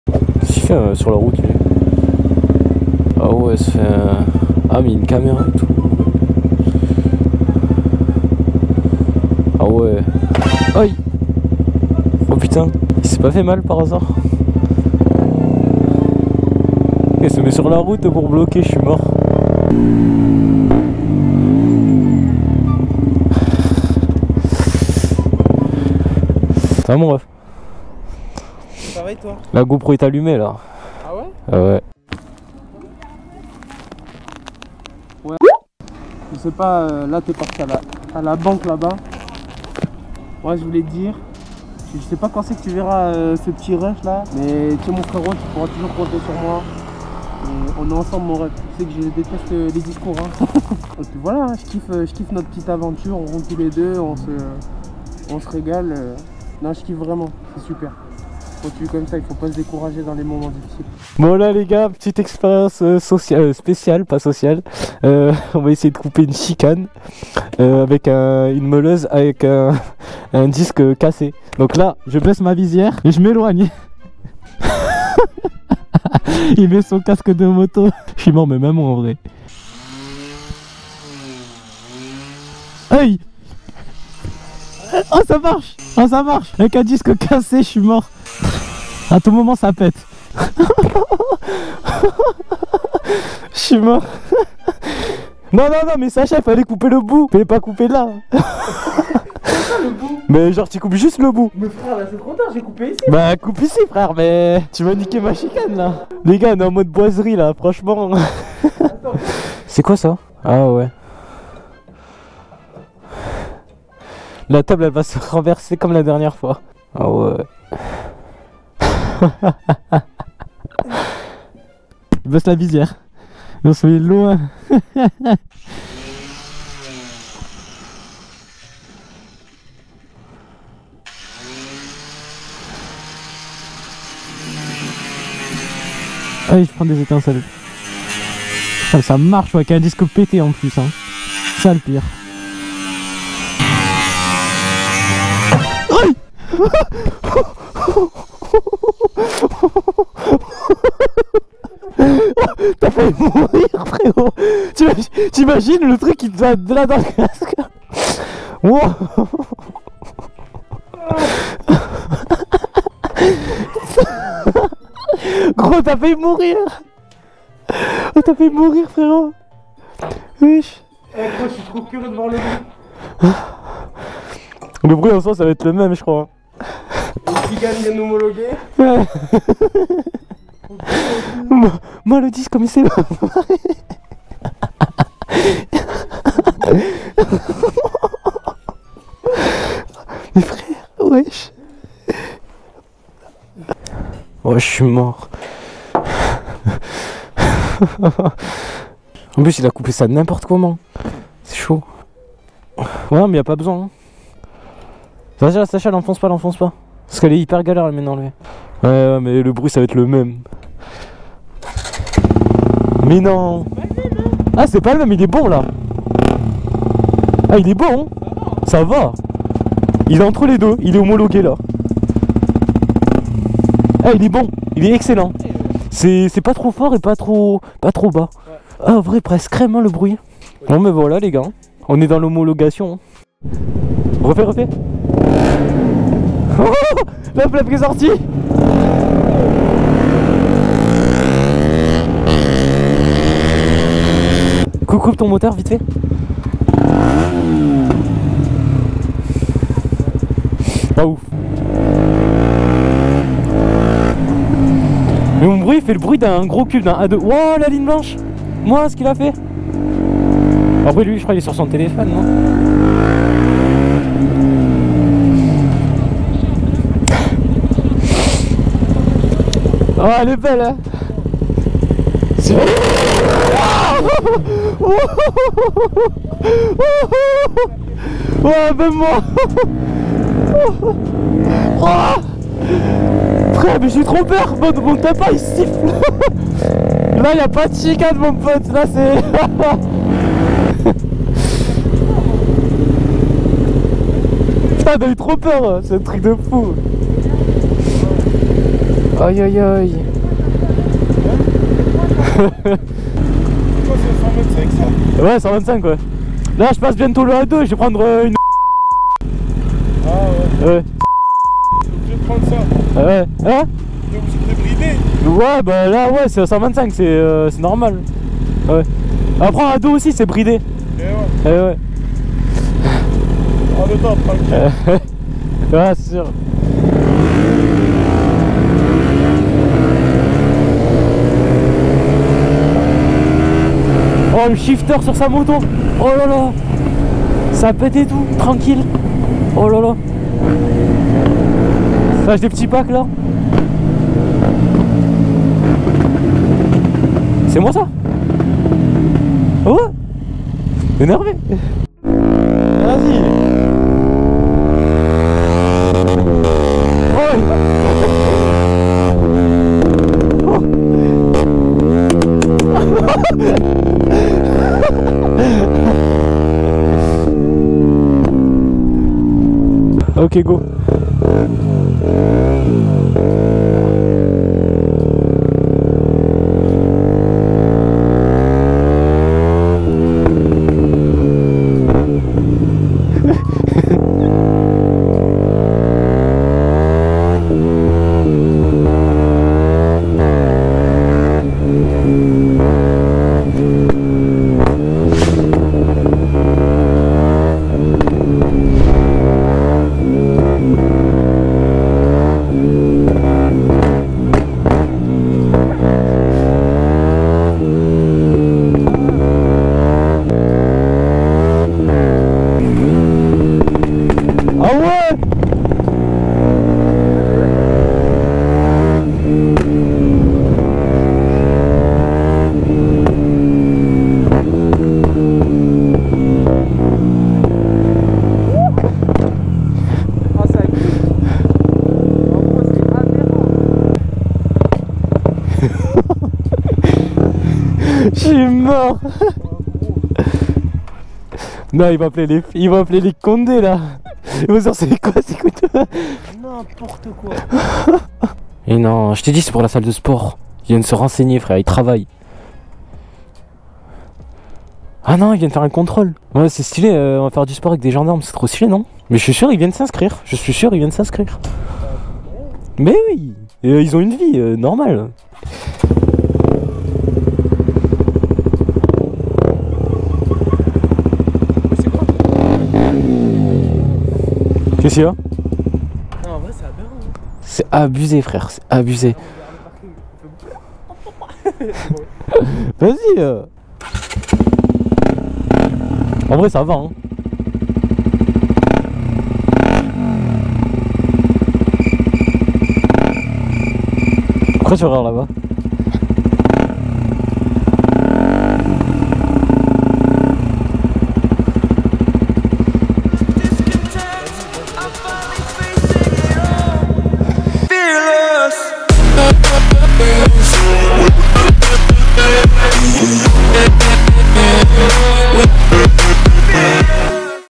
SON S1000RR NOUS A BRISÉ LES OREILLES 😱 (on a failli mourir)
IL NOUS A BRISÉ LES OREILLES 😱 (on a failli mourir)
GROS CONCOURS AUX 100K ! (une moto ?)

▬▬▬▬▬▬▬▬▬▬▬▬▬▬ LIENS RÉSEAUX  🤳🏼 ▬▬▬▬▬▬▬▬▬▬▬▬▬▬

Biker Denim 🐊